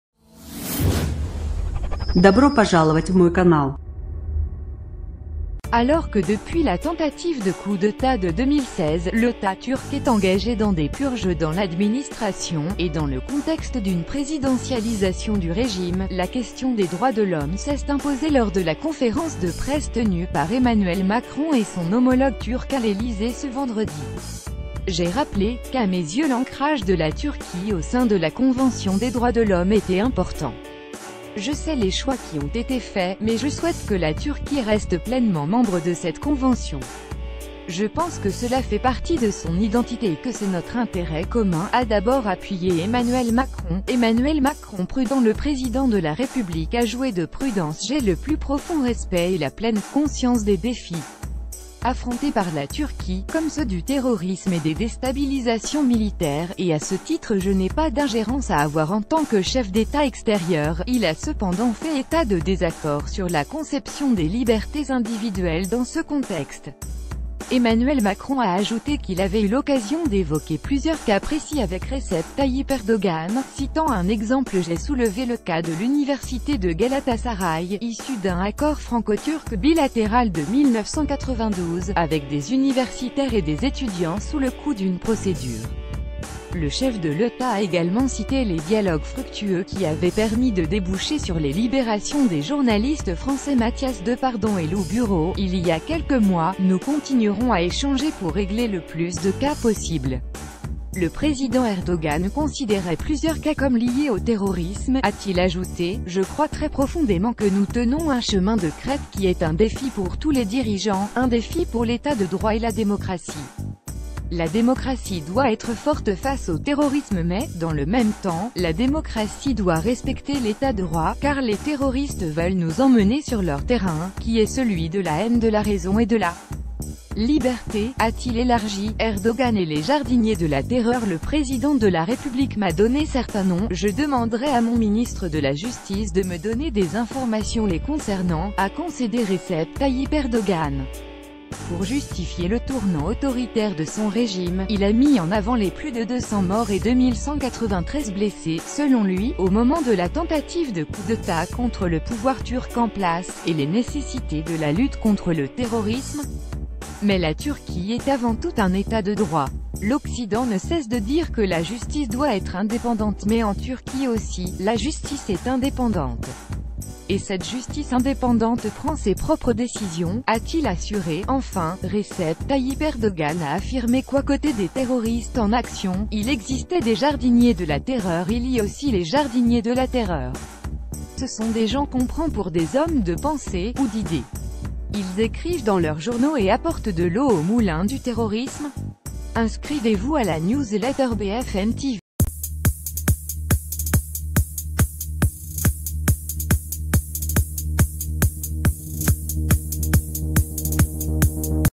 Macron évoque des « désaccords » sur les droits de l'homme avec erdogan
Alors que depuis la tentative de coup dEtat de 2016, lEtat turc est engagé dans des purges dans ladministration, et dans le contexte dune présidentialisation du régime, la question...